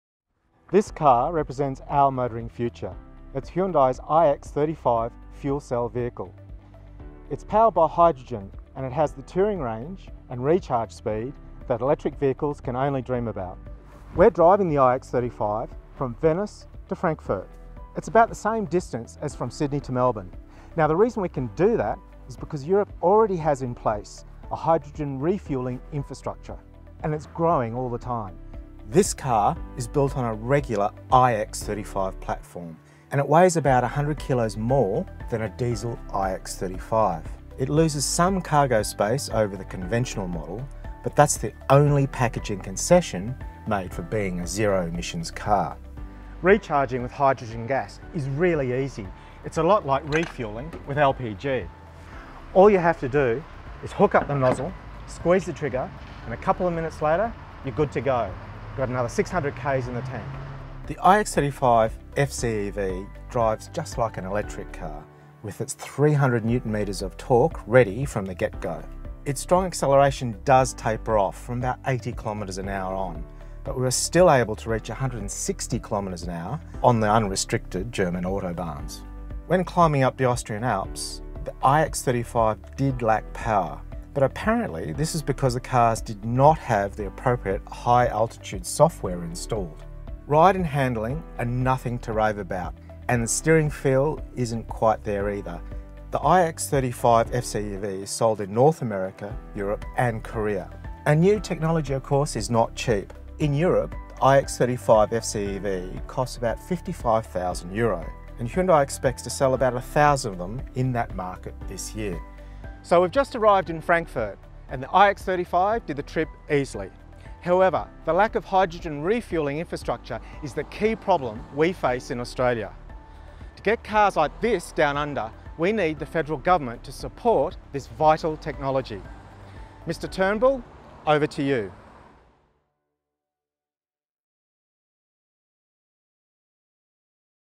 2015 Hyundai ix35 FCEV Review
Fuel cells use hydrogen to produce electricity via a chemical reaction – the only tailpipe emission is water vapour